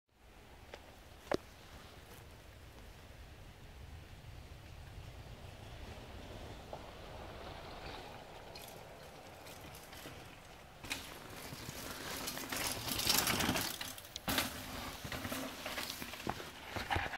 Little Larch Mountain Trail
Matt hitting the drop at the end of LLMT in Capitol Forest